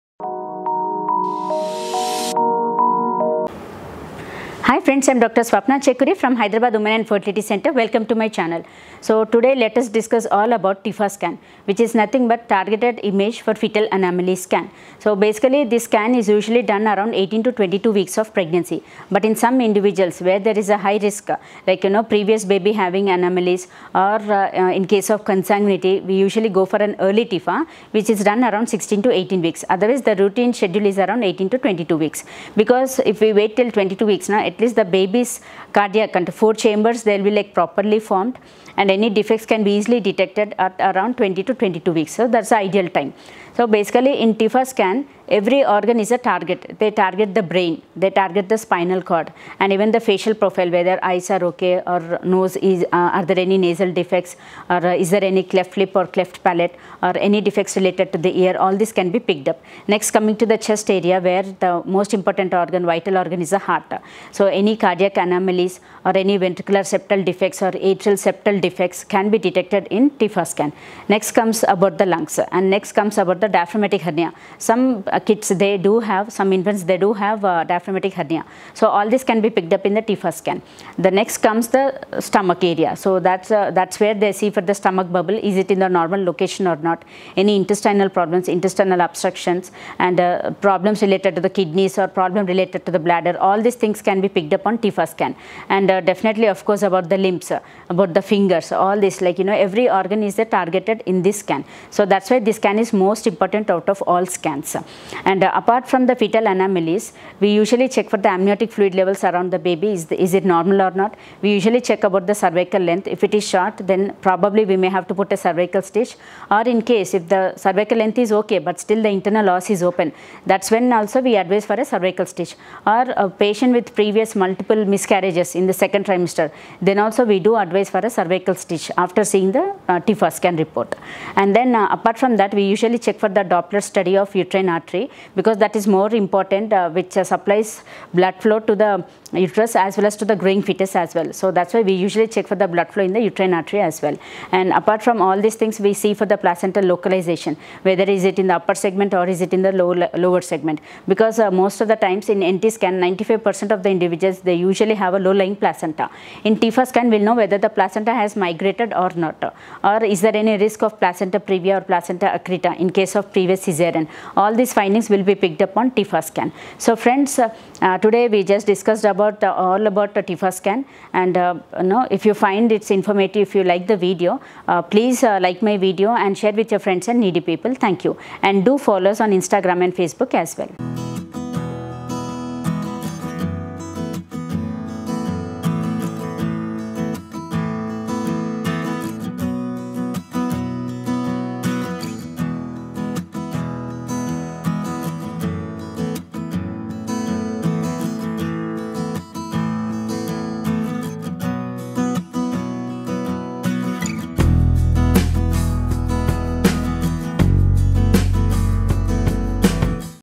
What is TIFFA Scan? - Importance of TIFFA In Pregnancy || Dr.Swapna Chekuri || HFC
A Tiffa (Targeted Imaging for Fetal Anomalies) scan is a specialized prenatal ultrasound that plays a crucial role in monitoring the health and development of the fetus during pregnancy. Here's a detailed description of why a Tiffa scan is important:
🌟 Unlocking the Wonders of Tiffa Scan: Why It's Essential During Pregnancy 🌟
In the journey of pregnancy, knowledge is power, and a Tiffa scan is a powerful tool that ensures the well-being of both mother and baby. Join us as we delve into the significance of this specialized prenatal ultrasound, uncovering why it's an essential milestone in the pregnancy care continuum.

A Tiffa scan is designed to provide detailed insights into the fetal anatomy and development. It allows healthcare providers to carefully examine the baby's organs, limbs, and overall growth, offering a comprehensive view that traditional ultrasounds might not provide. This advanced level of imaging helps detect any potential abnormalities or congenital anomalies early on.

Early detection is a key advantage of the Tiffa scan. By identifying any issues in the fetus's development, medical professionals can initiate timely interventions or treatment plans, ensuring the best possible outcome for both mother and baby. It's a proactive approach to safeguarding the health of your little one.

This video will guide you through what to expect during a Tiffa scan, emphasizing its non-invasive and safe nature. We'll also discuss the peace of mind it brings to expectant parents, as they gain a deeper understanding of their baby's progress.

Join us in celebrating the remarkable capabilities of modern medicine and the Tiffa scan's role in promoting healthy pregnancies. Informed choices lead to confident and joyful journeys to parenthood, and the Tiffa scan is a crucial part of that process.

TIFFA scan can tell us are:
Internal organs growth
Monitor the fetal movement.
Detect any birth defects.
Check the level of amniotic fluid.
Chromosomal abnormalities.
Check the position of the placenta.